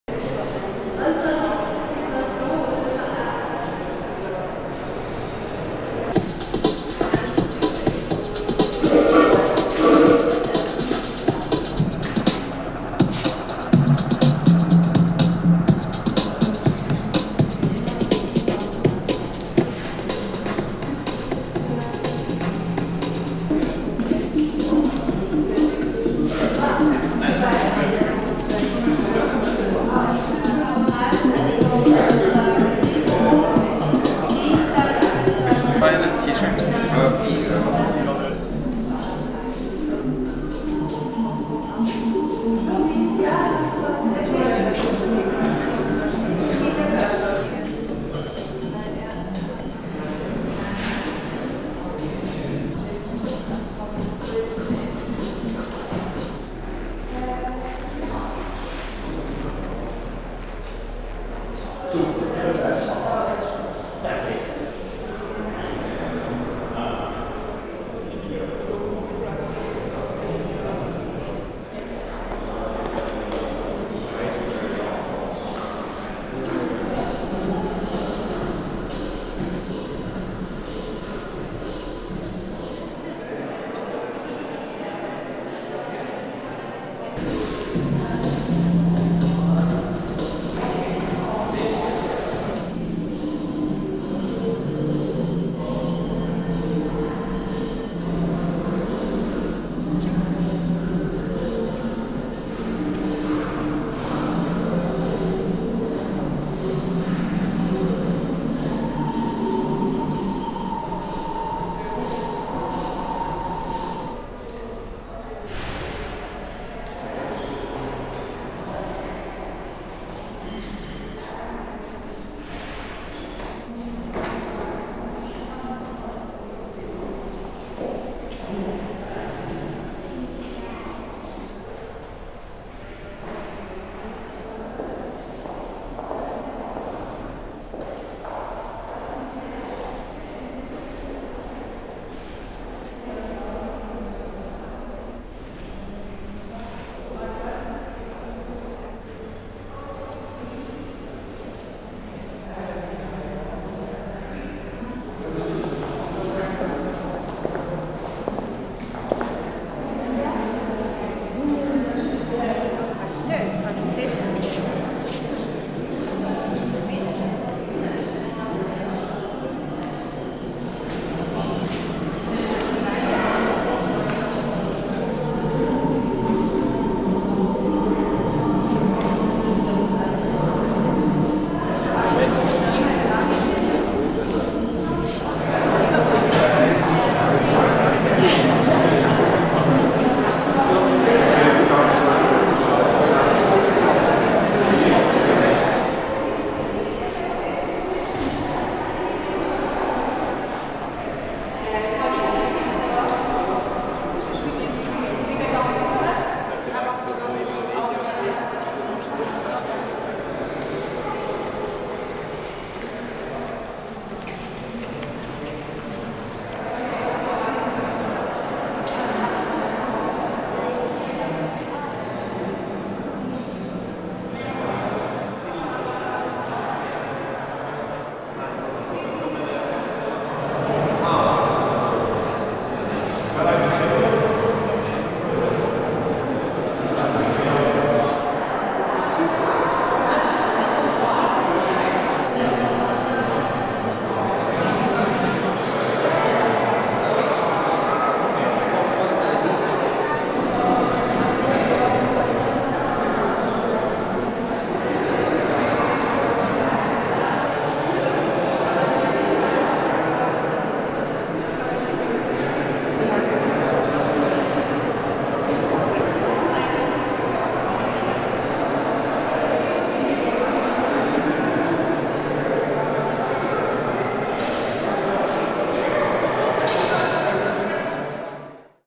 Ida Ekblad: FRA ÅRE TIL OVN / Kunsthalle Zürich
FRA ÅRE TIL OVN is Ida Ekblad's largest solo exhibition to date and presents her most recent works specially created for Kunsthalle Zürich. The title of the exhibition, derives from the book Norsk Jernskulptur. Norske Minnesmerker published in 1944. From Vein to Furnace is a short description of how metal is extracted from ore and cast into sculptures. The exhibition at Kunsthalle Zürich is divided into three chapters: a shop, a sculpture hall, and a huge space with large scale paintings.

Ida Ekblad (*1980) lives and works in Oslo. She studied at Central Saint Martins in London, the National Academy of Art in Oslo, and at the Mountain School of Arts in Los Angeles. She participated in the Venice Biennale (2011, 2017) as well as in numerous international group exhibitions.

Ida Ekblad: FRA ÅRE TIL OVN / Kunsthalle Zürich. Vernissage, June 7, 2019. (Zurich Art Weekend 2019)